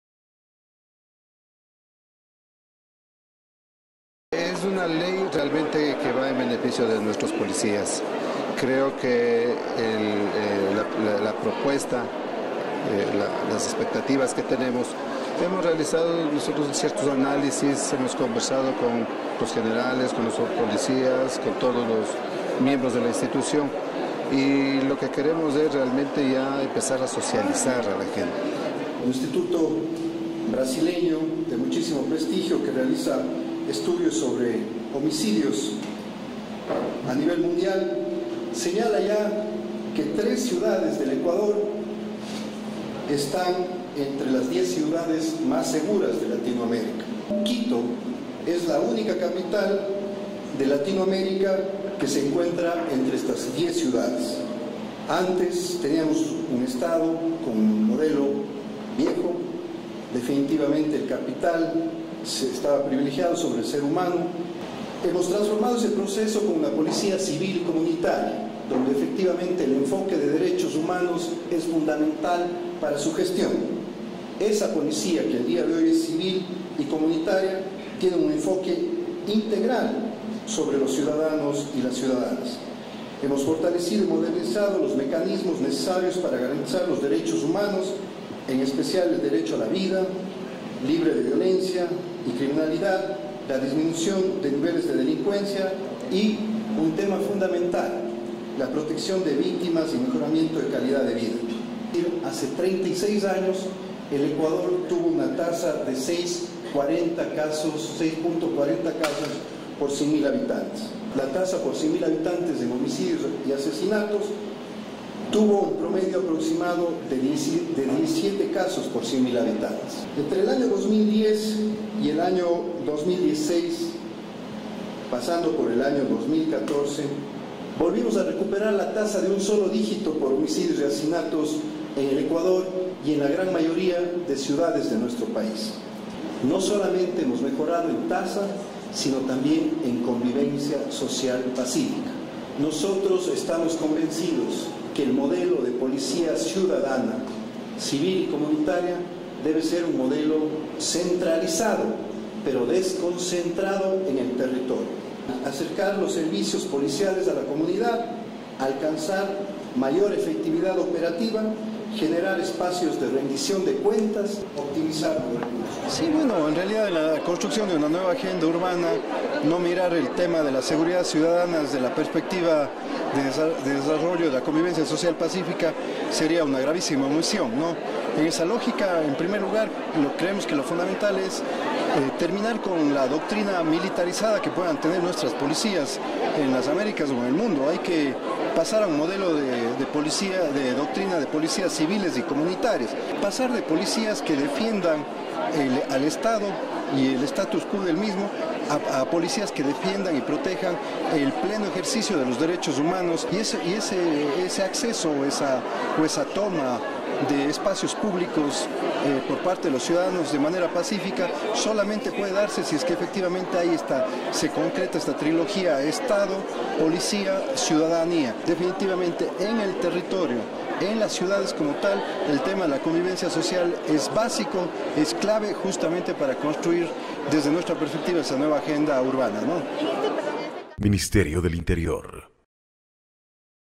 La seguridad ciudadana es pilar fundamental para el desarrollo urbano
Quito. La mañana de este 17 de septiembre se elevaron las banderas de la Organización de Naciones Unidas (ONU), en la Casa de la Cultura, marcándola como área para la realización de la Conferencia mundial sobre Vivienda y Desarrollo Urbano Sostenible Hábitat III. Este encuentro se desarrolla cada 20 años y en el 2014, Quito fue designada como sede del evento, por “su alta exposición turística a nivel mundial, sus recientes reconocimientos y su gran representación internacional”, según la revista Hábitat III.

En este primer día de conferencias, José Serrano, ministro del Interior, realizó una extensa exposición sobre el trabajo que viene realizando la Policía Nacional en pro de la convivencia social y pacífica, siendo este el gran aporte que logró ubicar a la capital de los ecuatorianos dentro de las seis ciudades más seguras de Latinoamérica, según un estudio realizado por el Instituto Igarapé, de Brasil.